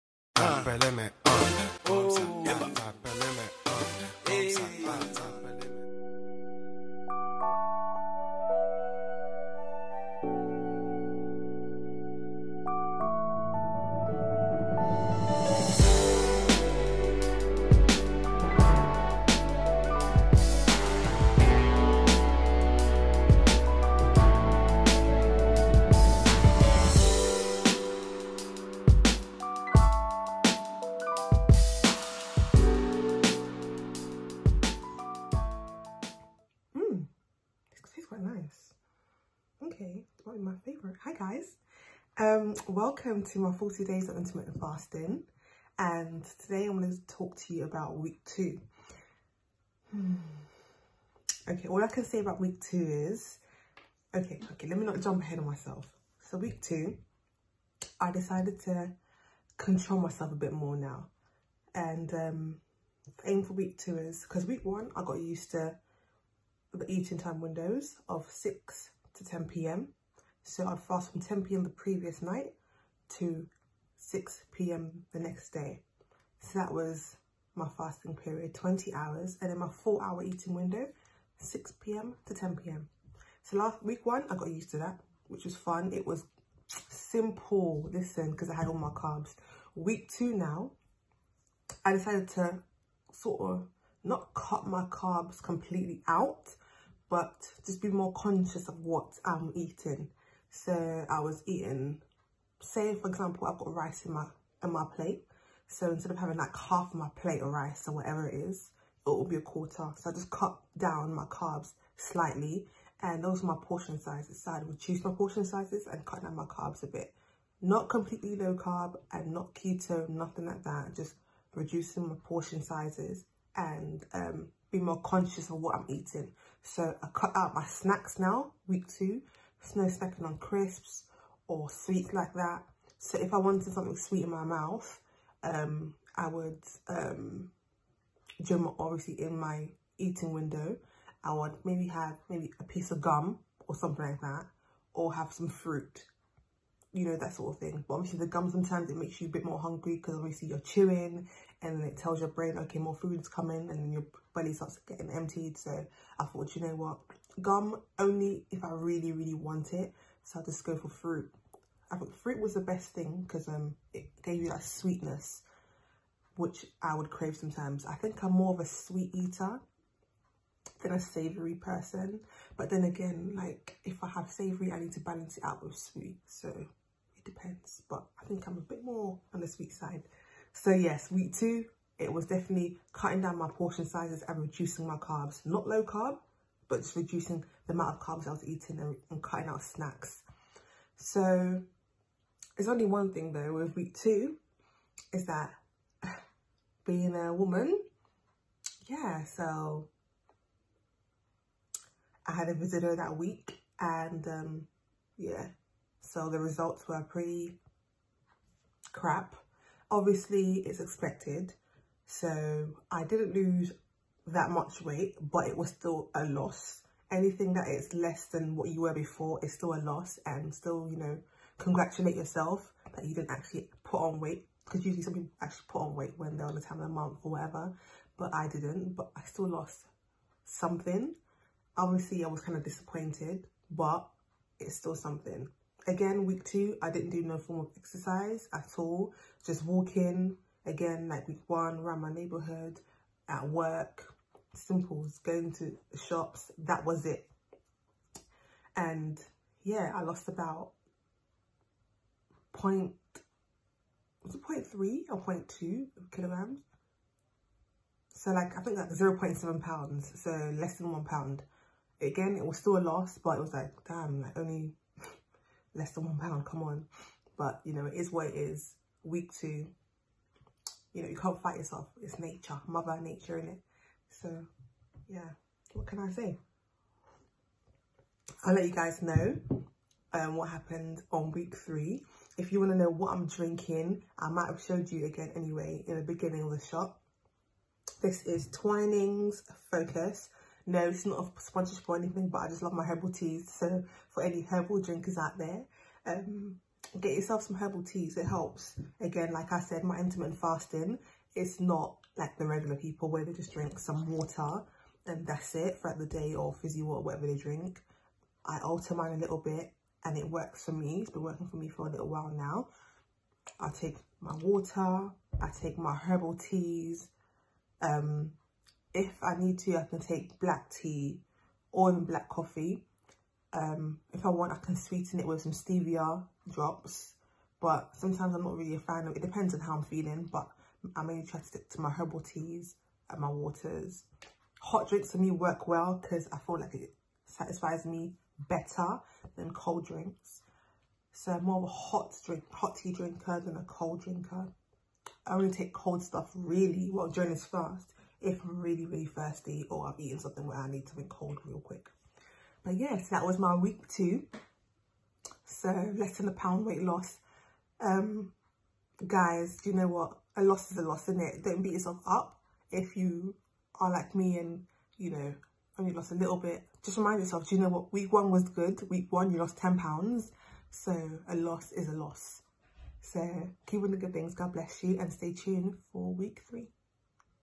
40 days Intermittent Fasting during LOCKDOWN | WEEK 2 of 5 | The Struggle!
I decided to try out 40 days of Intermittent fasting and see what the results were going to be. This is WEEK 2 and the struggle is real guys but... It is what it is!

Hair Instagram:  @ ComfortStyles
Health Journey Instagram: @ EnRouteFab